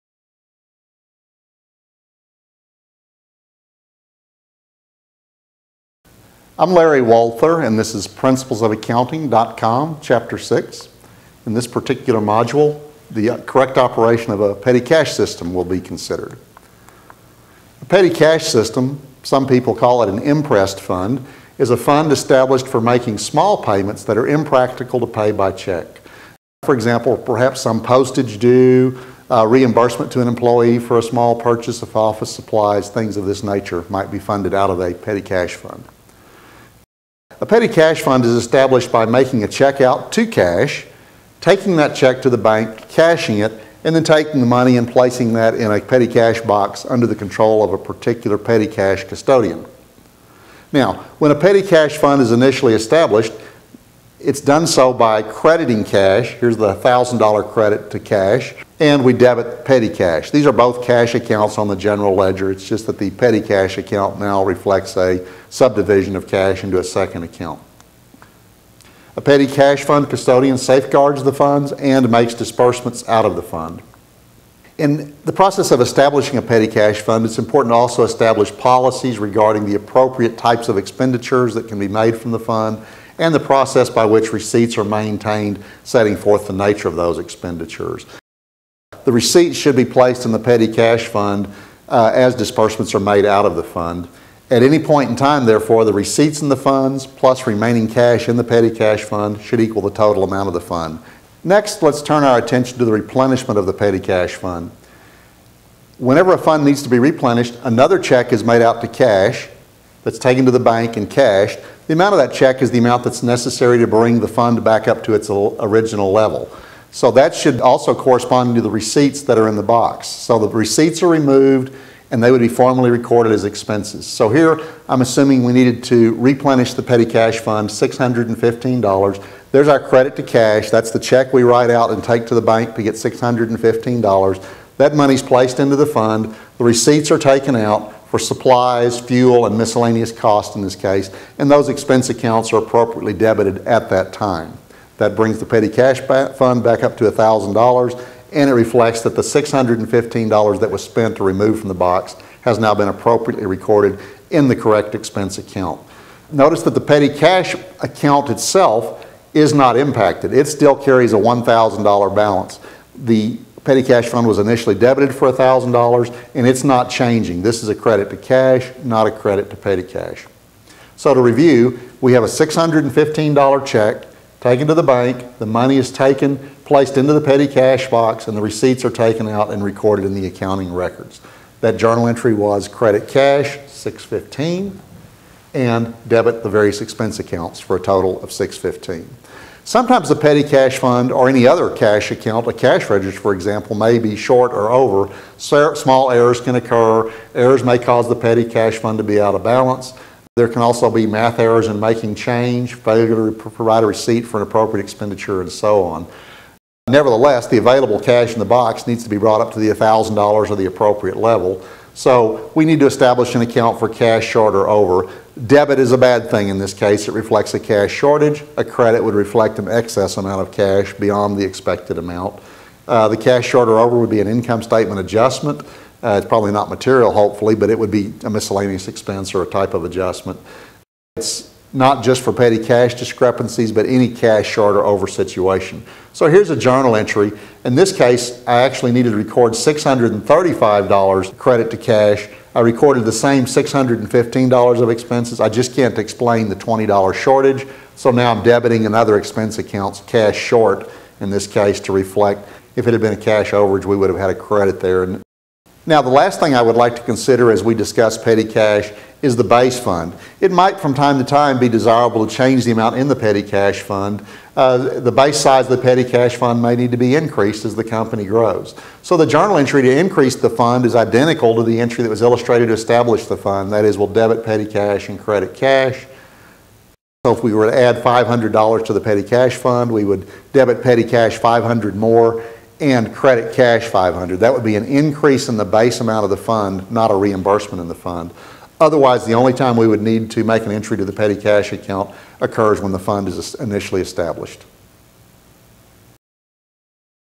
6 - A Petty Cash System
An overview of a petty cash system, to accompany Chapter 6, Cash and Highly Liquid Investments